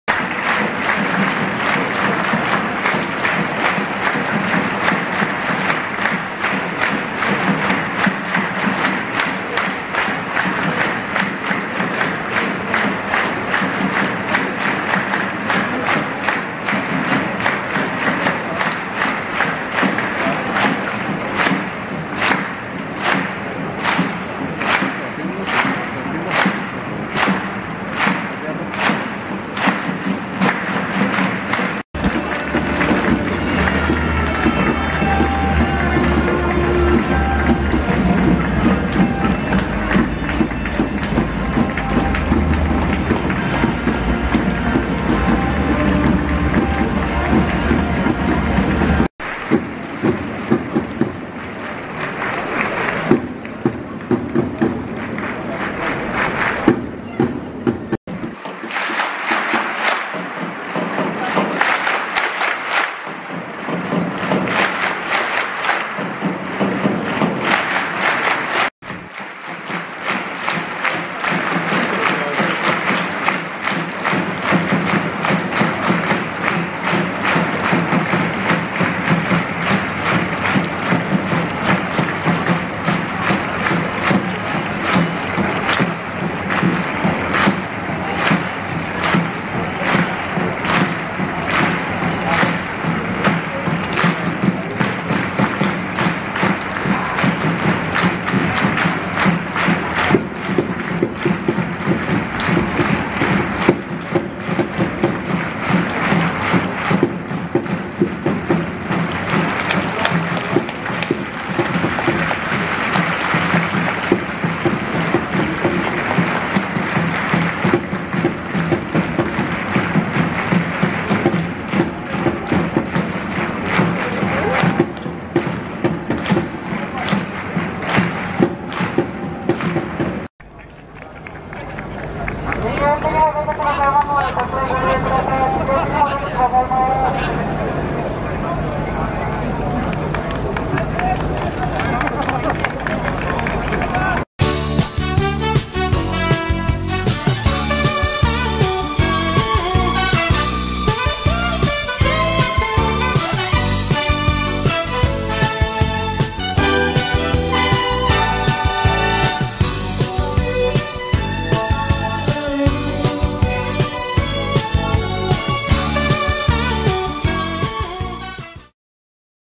【天皇杯】浦和レッズ、一席空け解禁後初の迫力のゴール裏の応援シーン。2021/12/12 天皇杯 浦和レッズーセレッソ大阪 埼玉スタジアム
2021/12/12 天皇杯 浦和レッズーセレッソ大阪 埼玉スタジアム